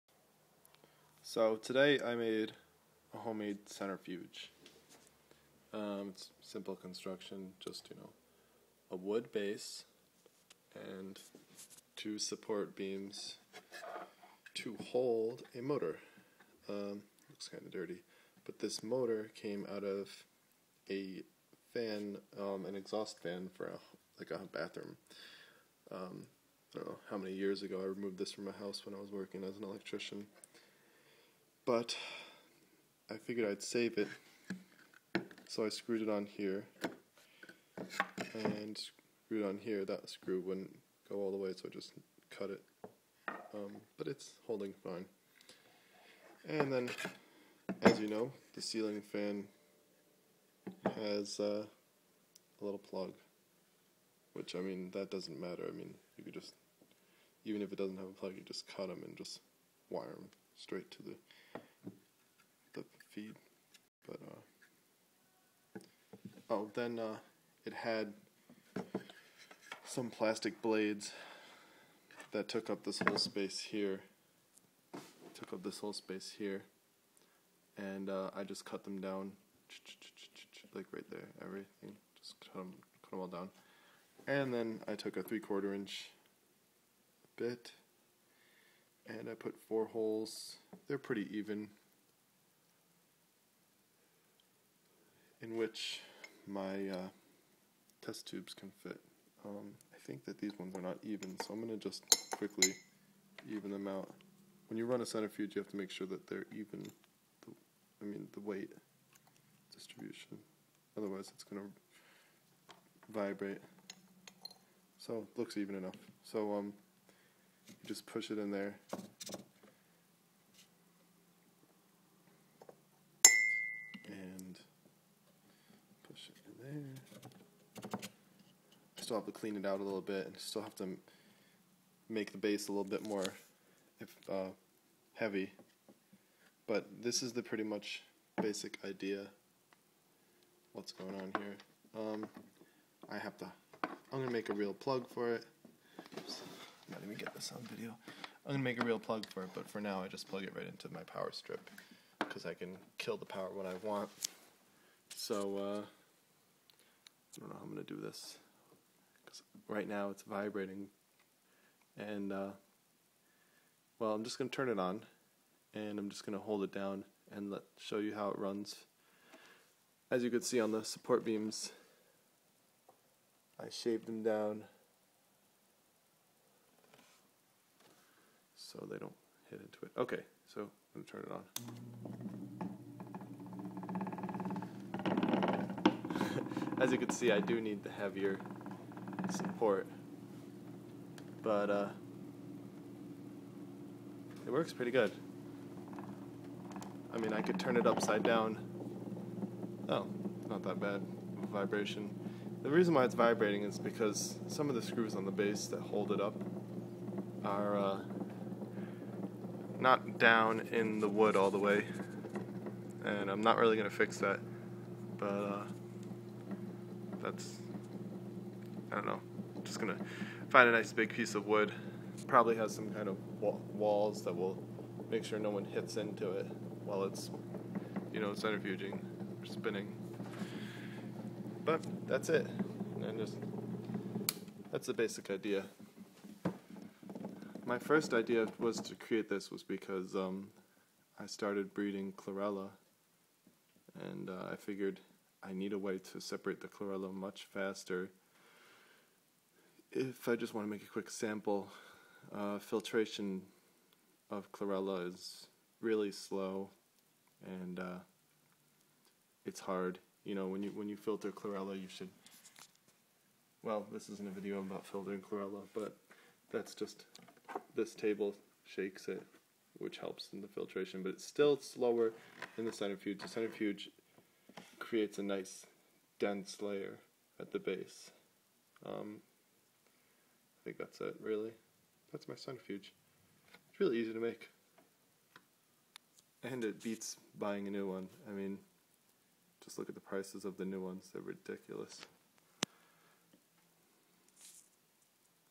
how to make a centrifuge at home
in this video i describe how i made my home made centrifuge. its simple science really.
the particles get pushed outwardly, and since the tube is on an angle, the particles travel downward towards the furthest path. which happens to be the base of my test tube.

this is good for my chlorella sciences.